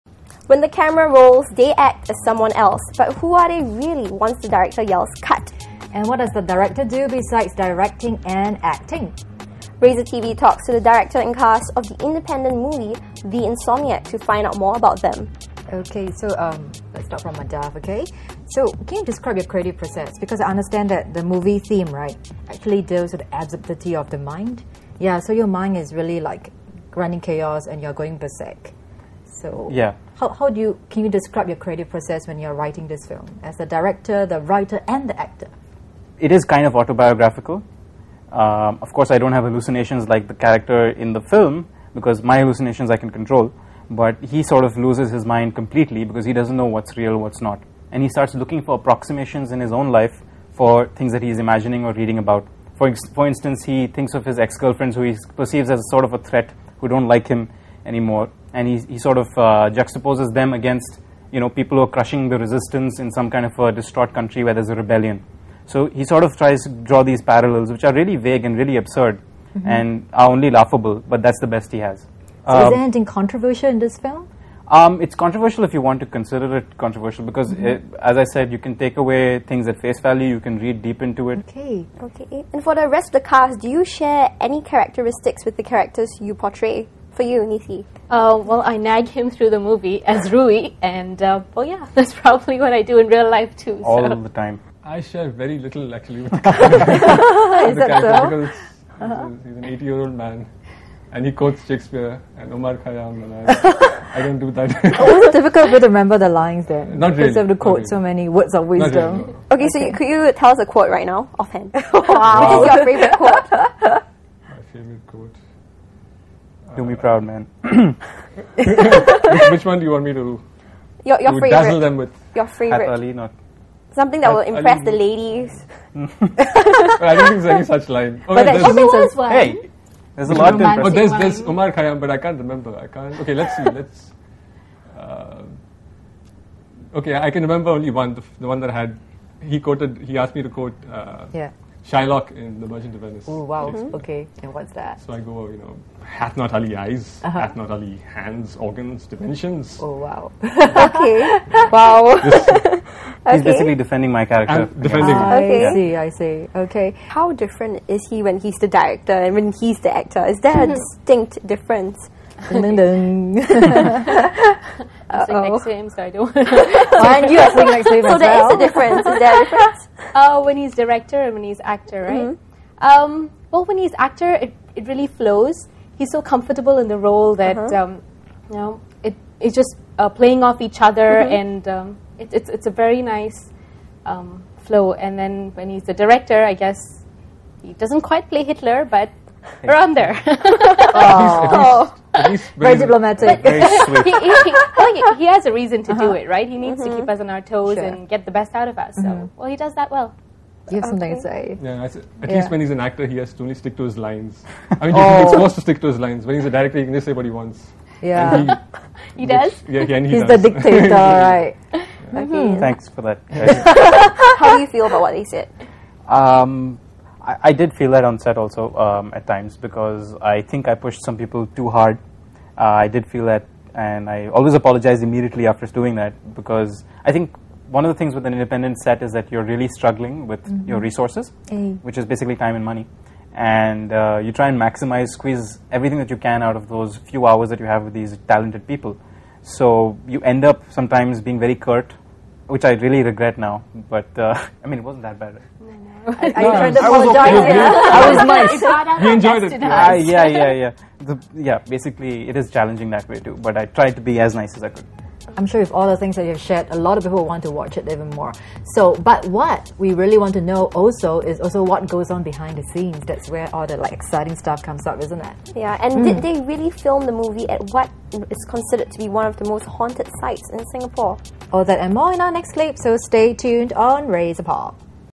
He Quotes Poetry! (SIF Pt 9)
On this Singapore Indie Film (SIF) special, we get real with the cast and director of The Insomniac and we discover one of the guys actually quotes poetry.